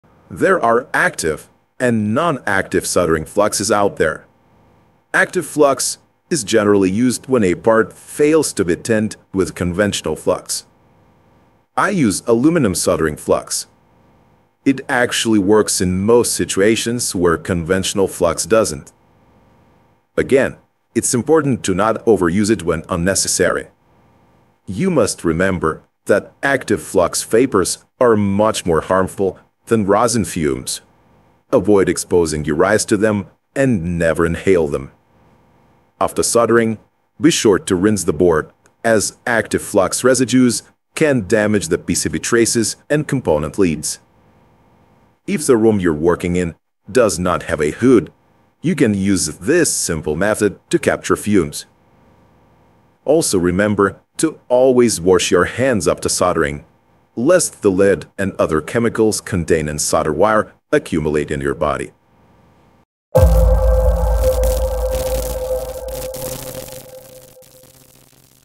Using active soldering flux. Rinsing the board.
BTC:384FUkevJsceKXQFnUpKtdRiNAHtRTn7SD
ETH: 0x20ac0fc9e6c1f1d0e15f20e9fb09fdadd1f2f5cd

There are active and non-active soldering fluxes out there. Active flux is generally used when a part fails to be tinned with conventional flux. I use aluminum soldering flux. It actually works in most situations where conventional flux doesn’t. Again, it’s important to not overuse it when unnecessary. You must remember that active flux vapors are much more harmful than rosin fumes. Avoid exposing your eyes to them and never inhale them. After soldering, be sure to rinse the board, as active flux residues can damage the PCB traces and component leads. If the room you are working in does not have a hood, you can use this simple method to capture fume. Also remember to always wash your hands after soldering, lest the lead and other chemicals contained in solder wire accumulate in your body.